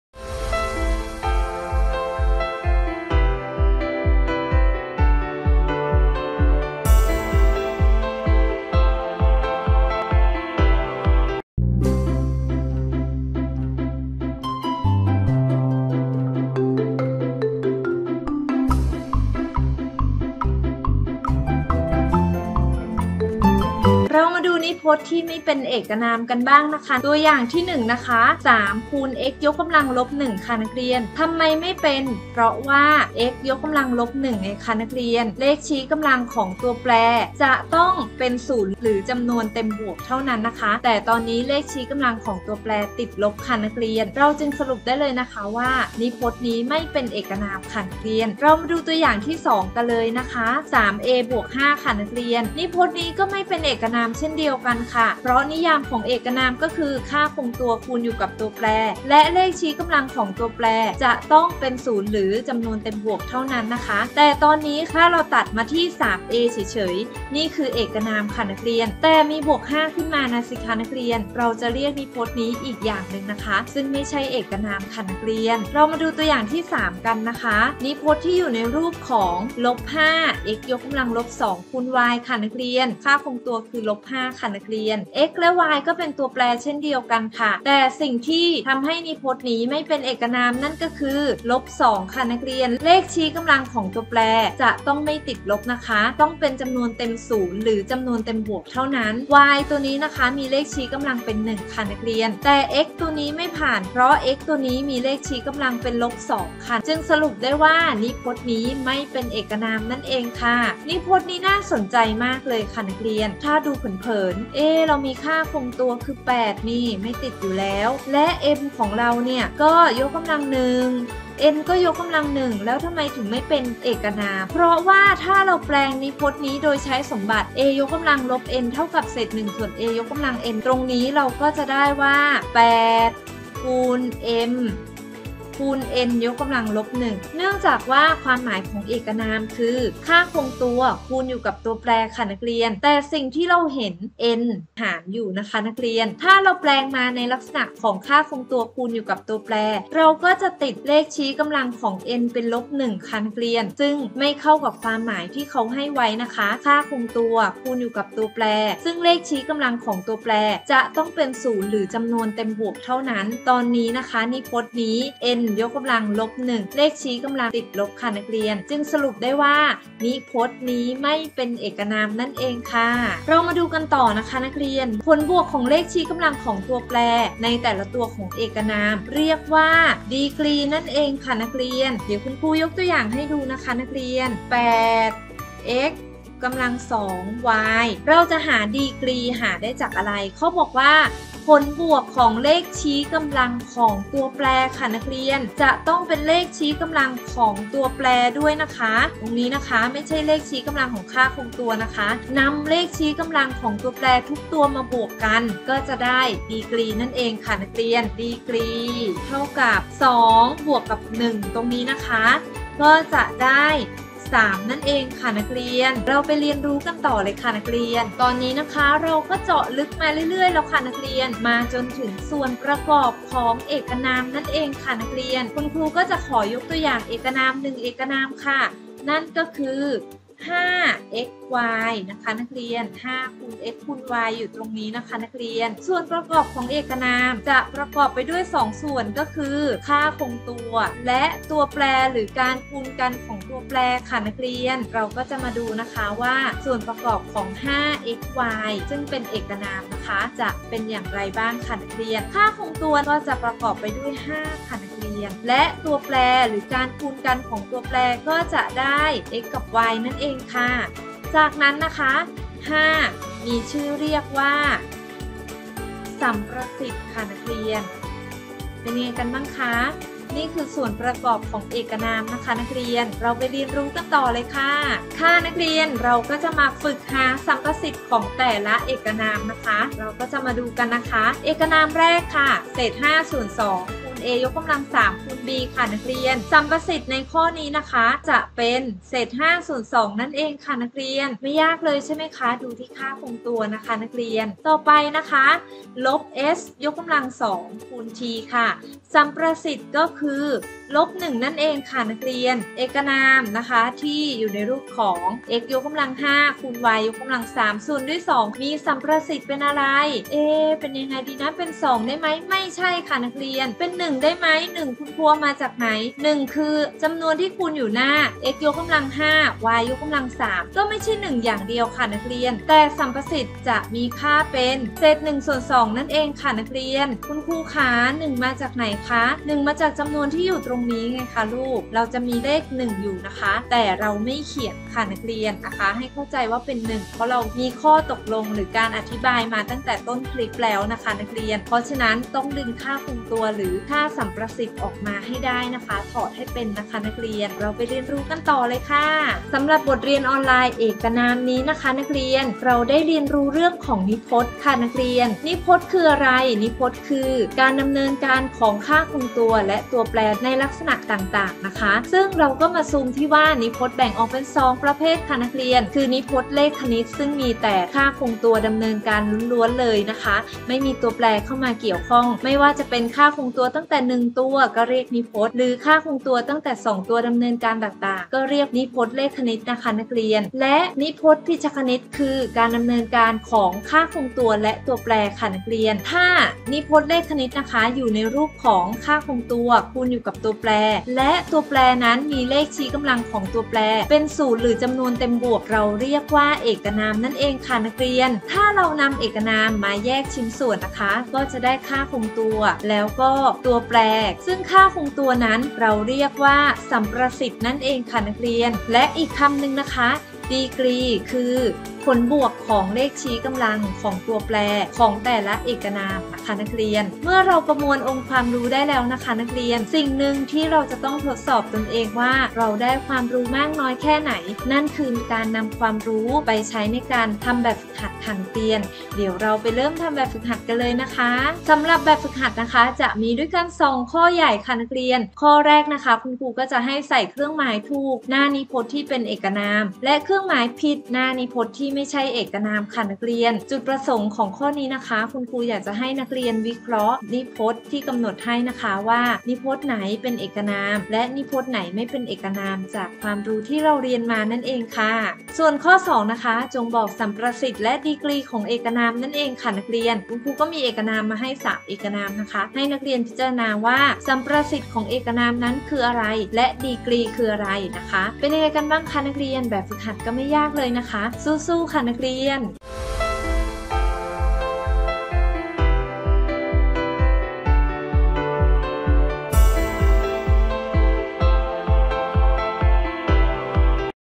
วิชาคณิตศาสตร์ ม.2 เรื่องเอกนาม ตอนที่ 2 สอนโดยครูสุภาพรรณ เอมสมบุญ
โรงเรียนนครไทย สหวิทยาเขตบางกลางท่าว
จังหวัดพิษณุโลก สำนักงานเขตพื้นที่การศึกษามัธยมศึกษาพิษณุโลก อุตรดิตถ์